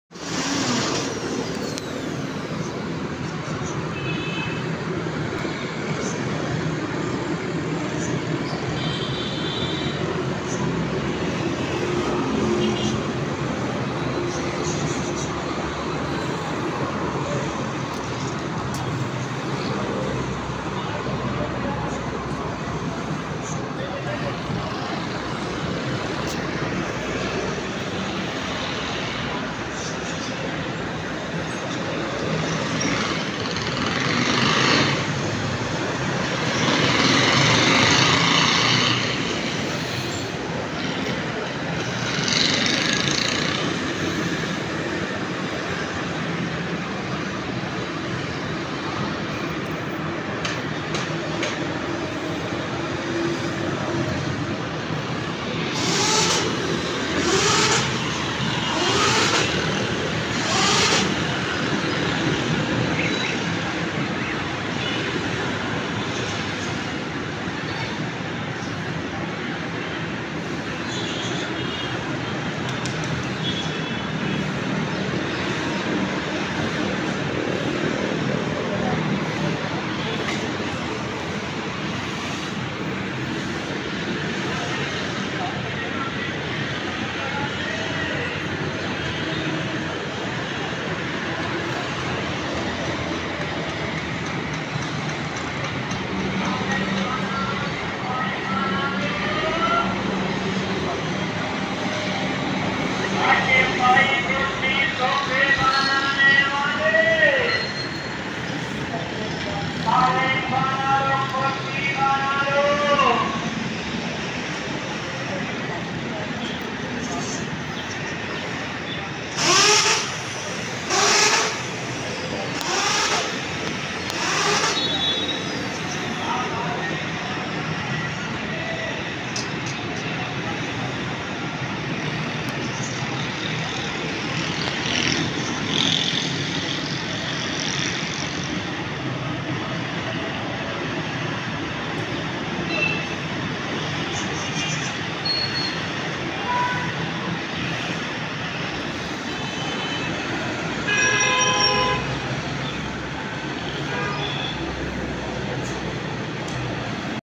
earth drilling machine for water 300 feet in deep bore Karachi chudai
earth drilling machine for water 300 feet in deep bore Karachi chudai or ... Kabhi nahi rukti